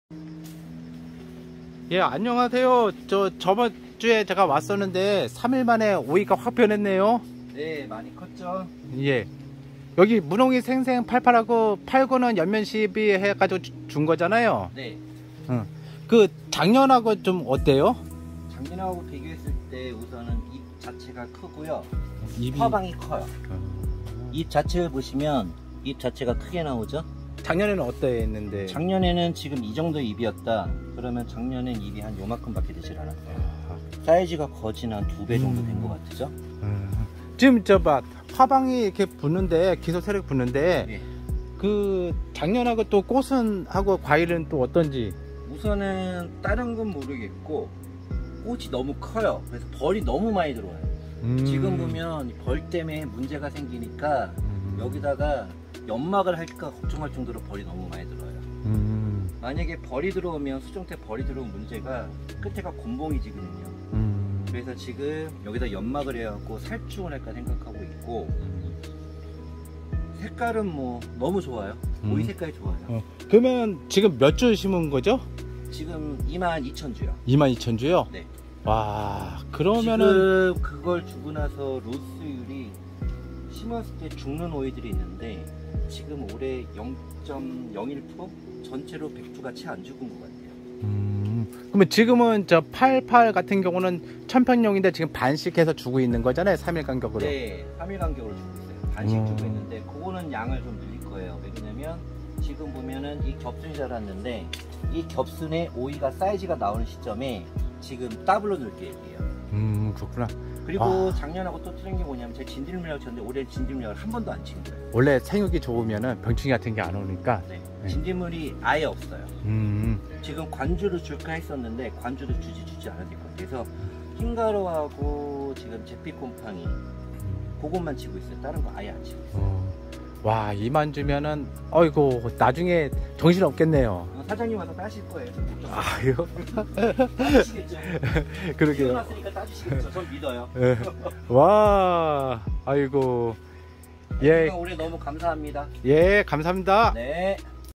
오이 2만2천주 터진다.
오이 2만2천주 출하합니다.

청오이 가시오이 2만2천주..

정식 후 무농이생생88를 3일간격으로..
무농이생생89는 농약과함께 엽면..

작년보다 잎이 커지고 꽃도 커지고..
물량이 터지면 박스작업이 걱정이라는..

무농이생생88과 무농이생생89는 미생물로 100프로 숙성되었기에 빠른흡수 빠른효능을 볼수있습니다

사람이 땅에서 생명을 키웁니다
농업회사법인 HSBIO